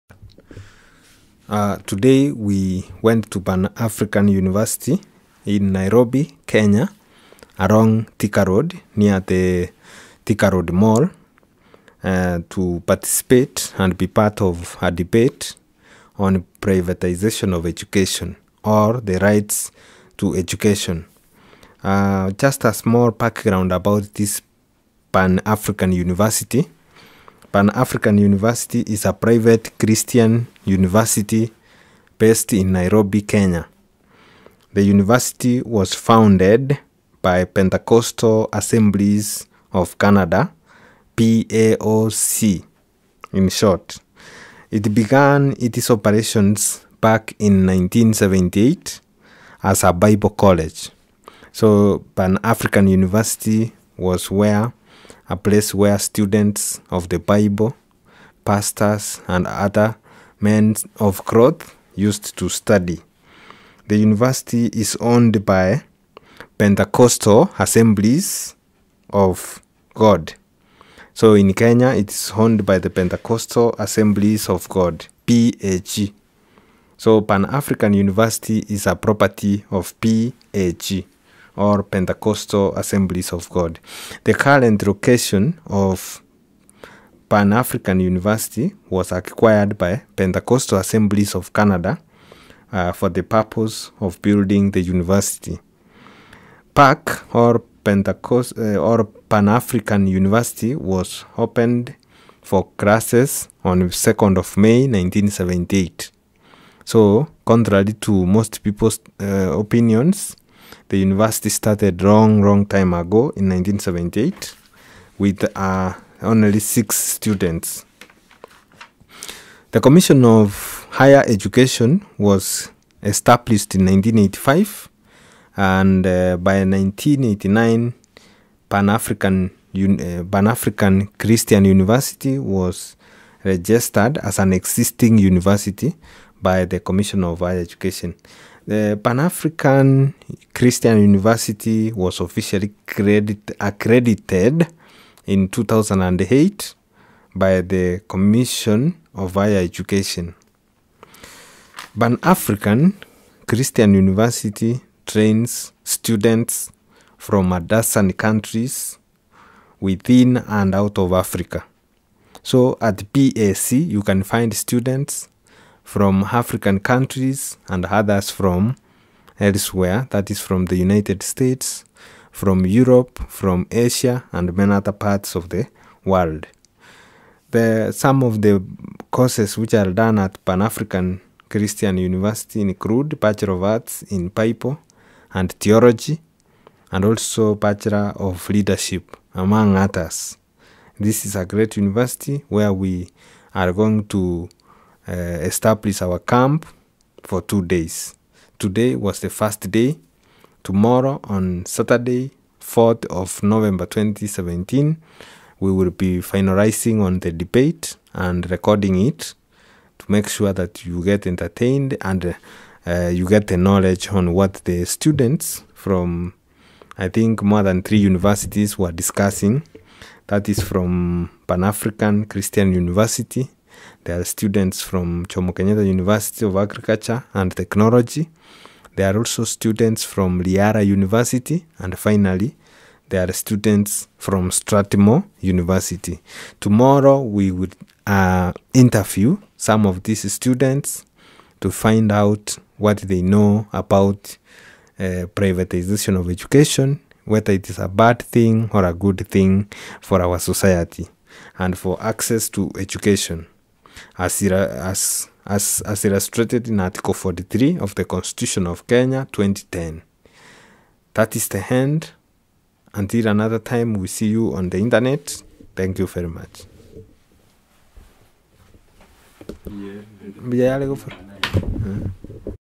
BIG DEBATE AT Pan African Christian University PAC
This is about the big debate organized by SCOHRA, Hakijamii, PAC University, Amnesty International among many others.